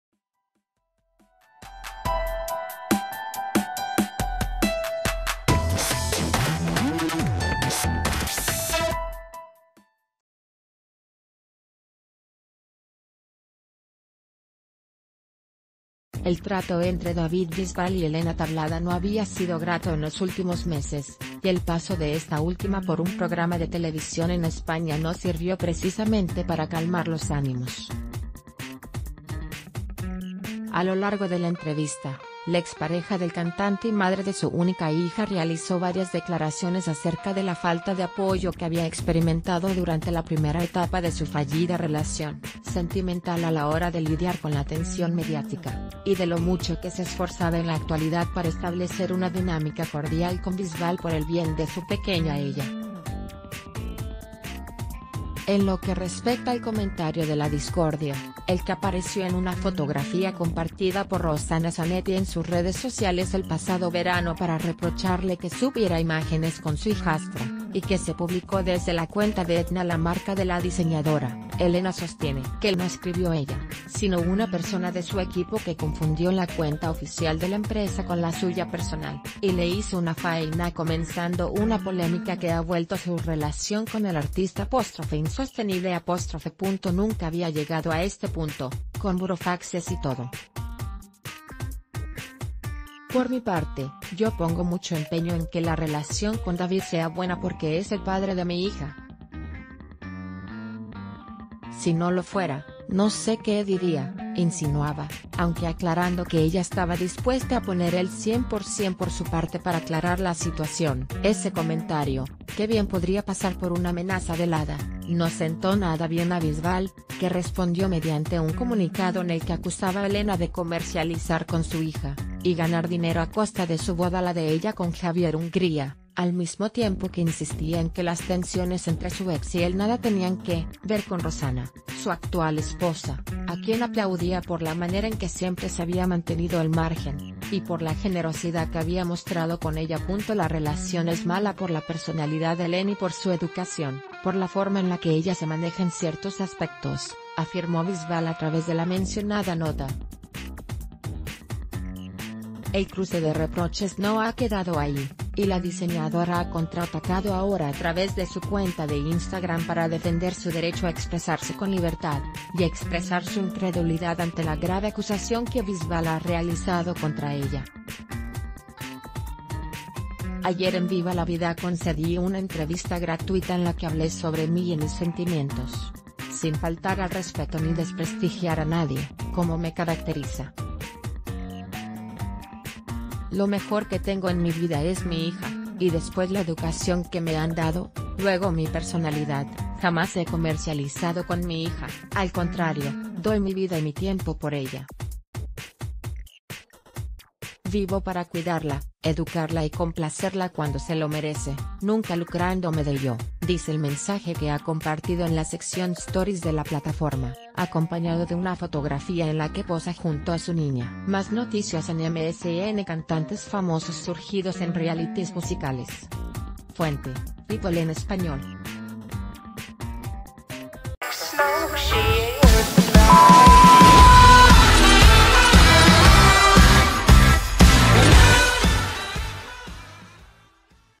David Bisbal acusa a su ex de "comercializar" con su hija para ganar dinero
El trato entre David Bisbal y Elena Tablada no había sido grato en los últimos meses, y el paso de esta última por un programa de TV en España no sirvió precisamente para calmar los ánimos. A lo largo de la entrevista, la expareja del cantante y madre de su única hija realizó varias declaraciones ac...